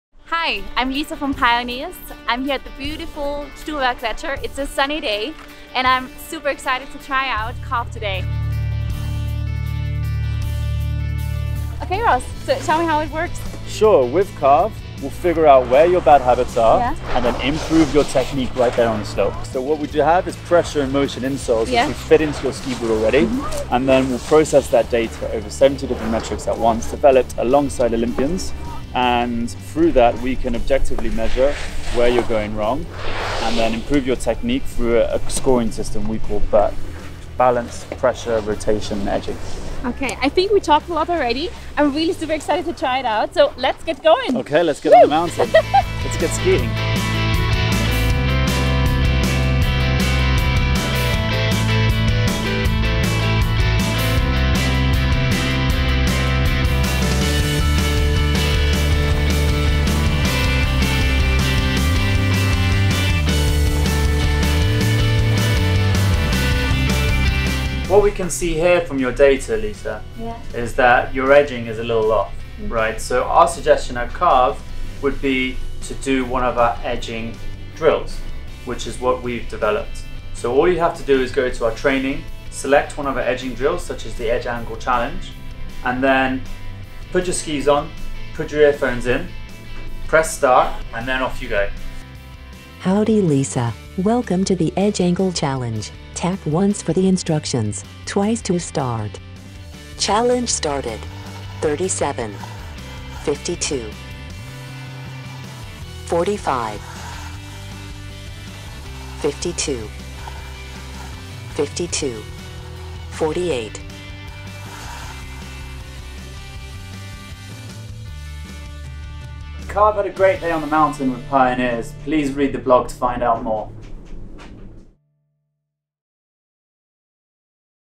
Pioneers Tests: Carv's Digital Ski Coach
We took a couple of keen Pioneers guinea pigs to an Austrian glacier, where they tried out the ‘digital ski coach’ developed by Pioneers’17 alumnus Carv. Watch for their findings on one of the most ambitious innovations to hit the slopes in a long time…